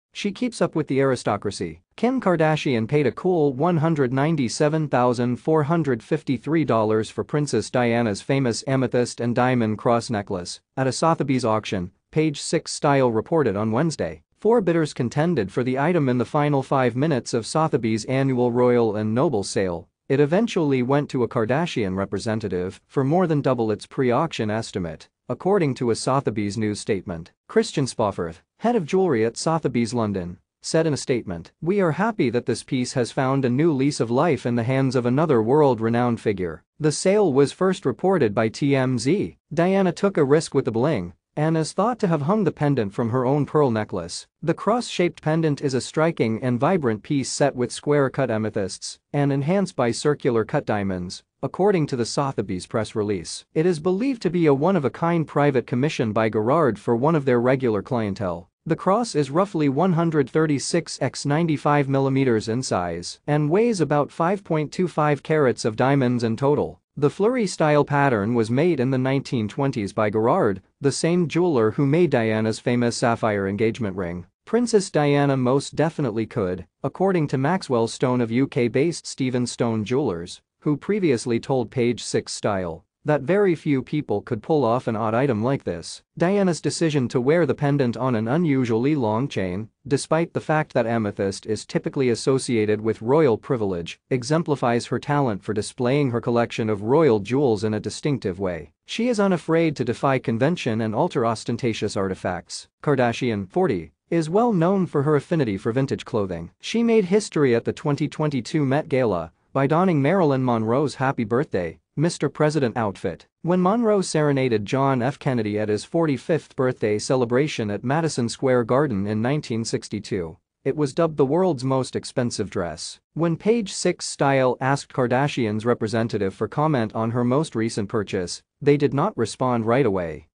Princess Diana's amethyst cross necklace is purchased by Kim Kardashian for $200,000.
She keeps up with the aristocracy.
Kim Kardashian paid a cool $197,453 for Princess Diana's famous amethyst and diamond cross necklace at a Sotheby's auction, Page Six Style reported on Wednesday.